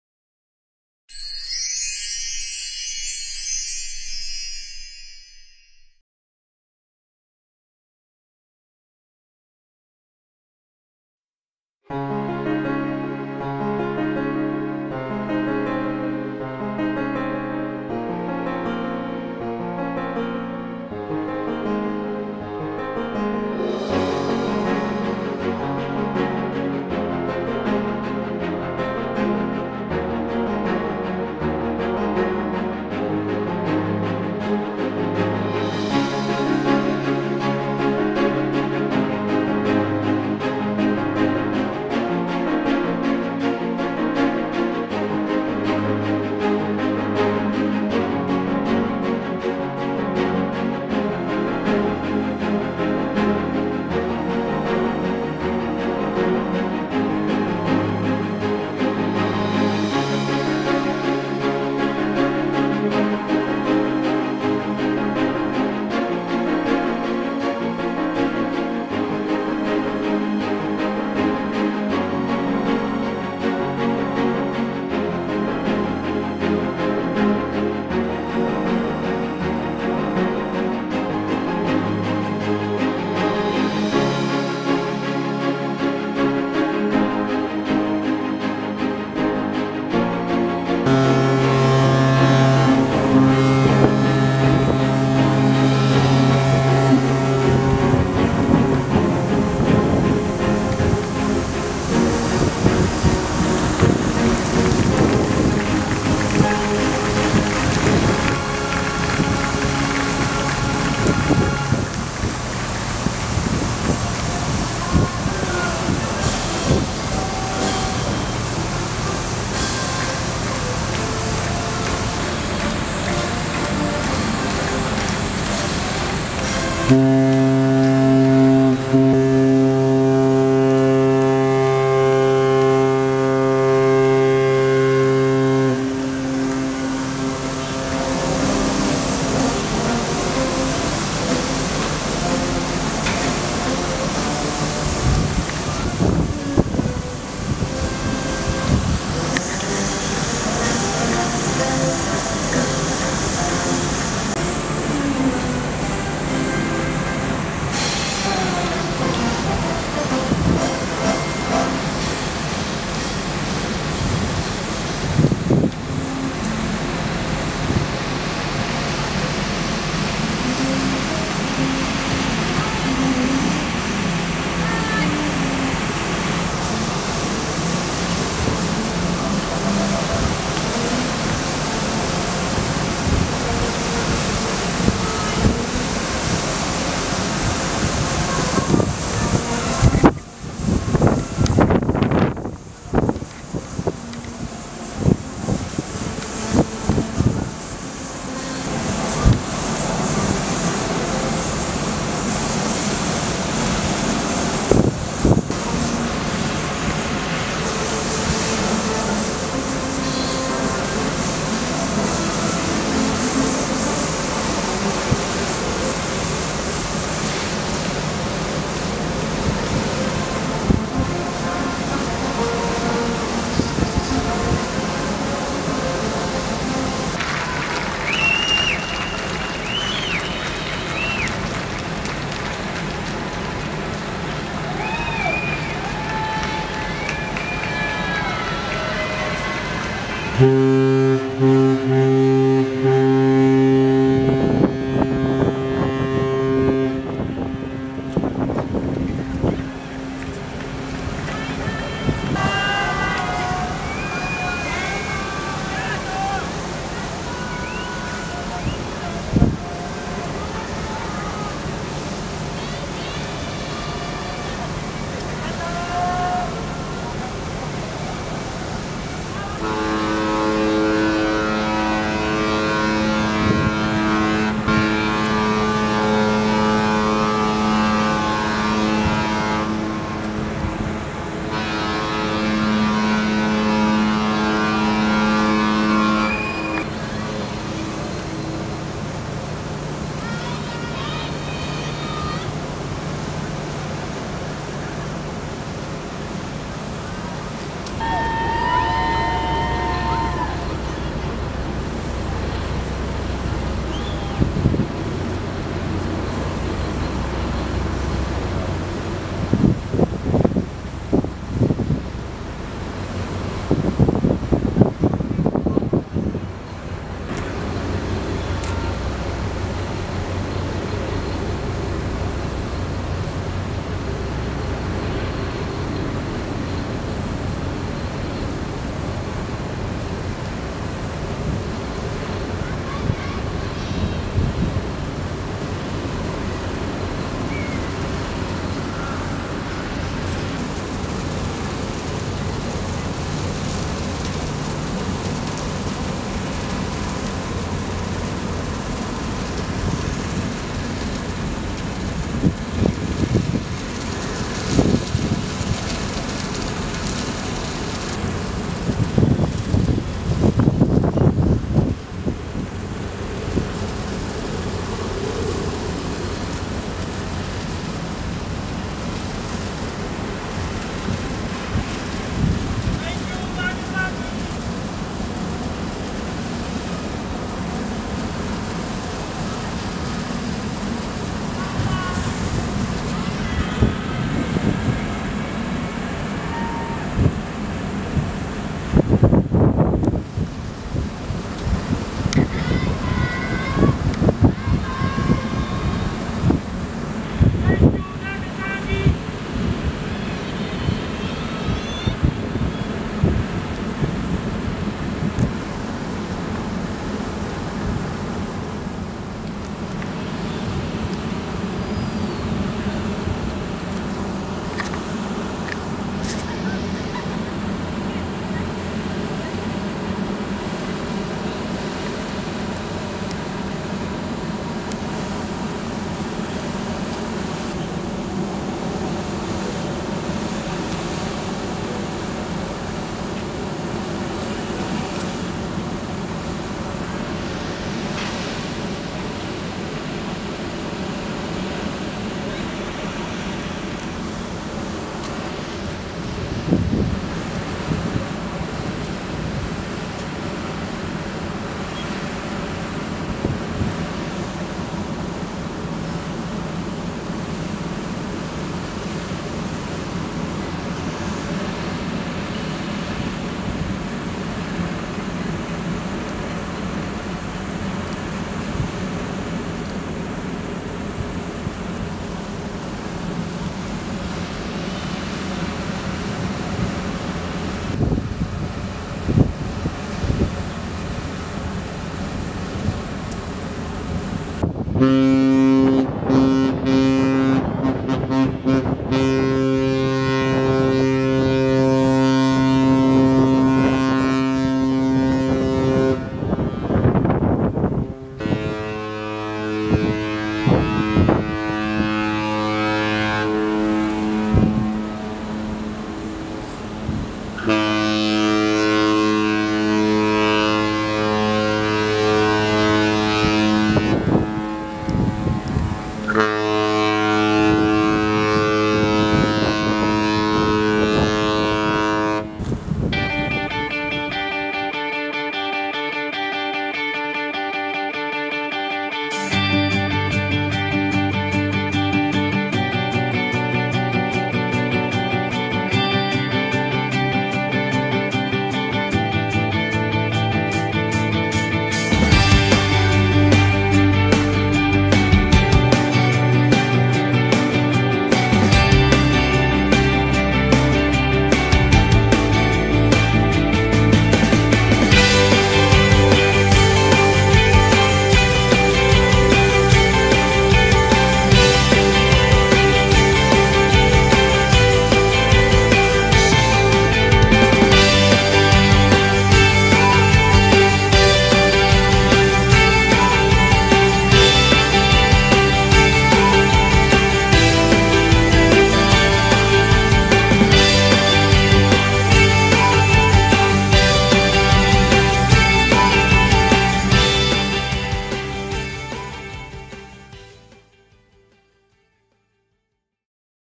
長崎港に２隻の豪華客船
７月１５日に同時２席の豪華客船が長崎港松ヶ枝岸壁・水辺の森に接岸した、２隻入港することは珍しく多くの見物人でにぎあった、同日５時と６時に次々と出港していった。